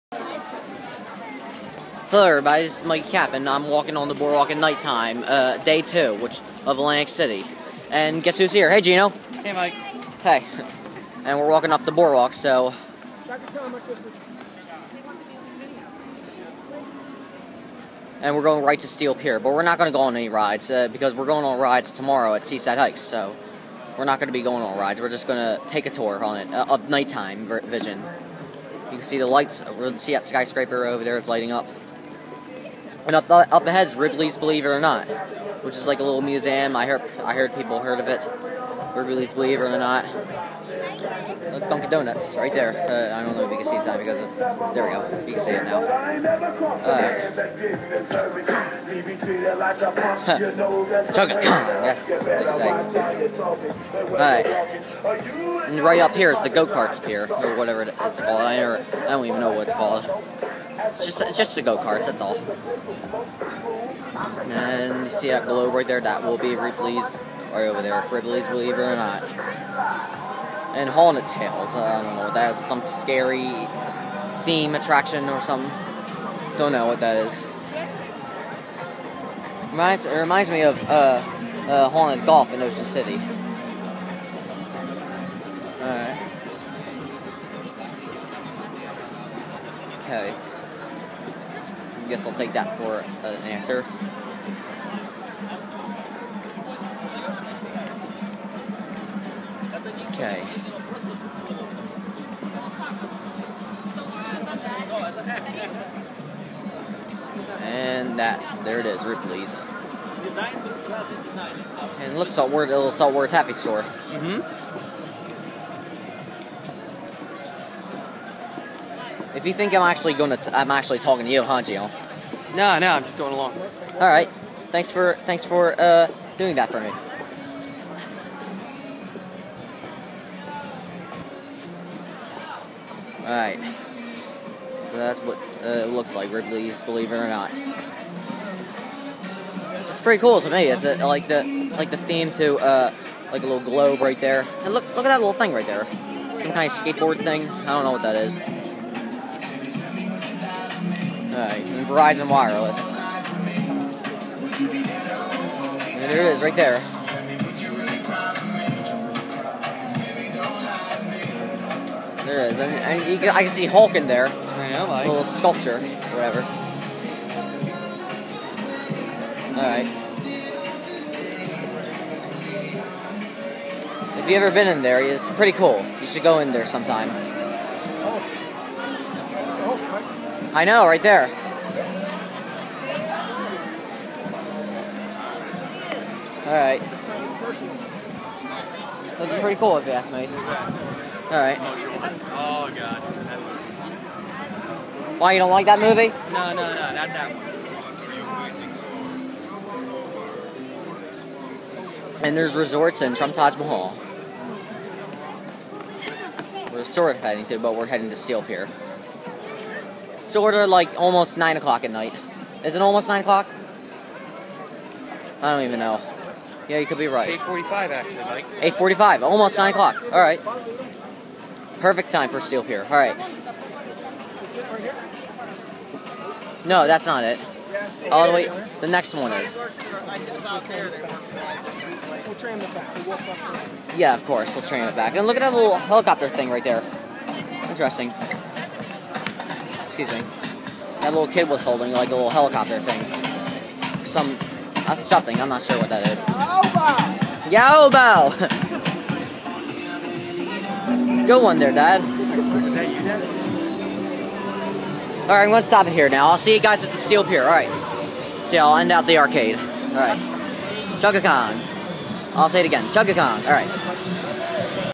Atlantic city Day 2 part 1 of 3 walking on Boardwalk night time
This is day 2 for ac and I'm on the boardwalk at night time!